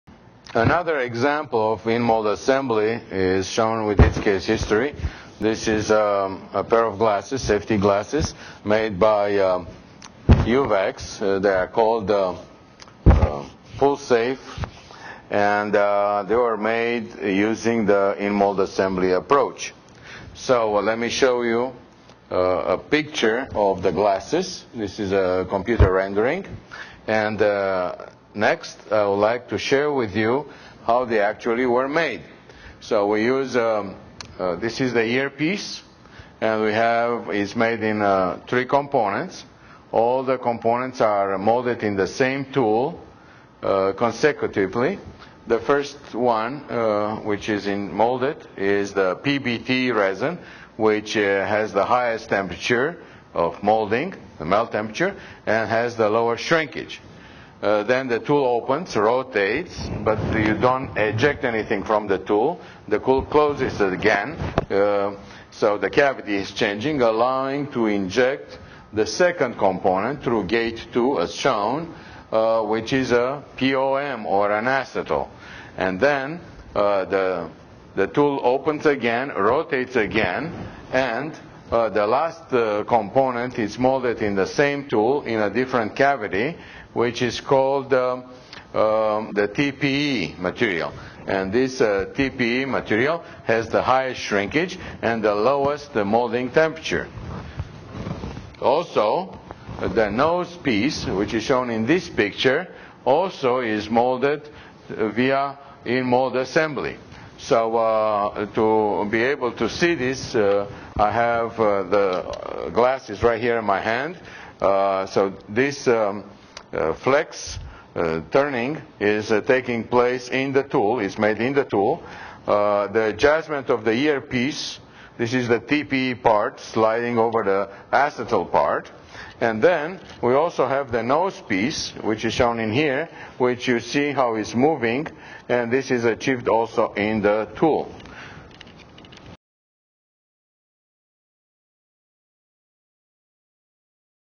In-Mold Assembly - Automotive Plastic Part Design at ETS Corp.
Paul Tres details cutting edge in-mold assembly techniques to streamline plastic part assembly time and costs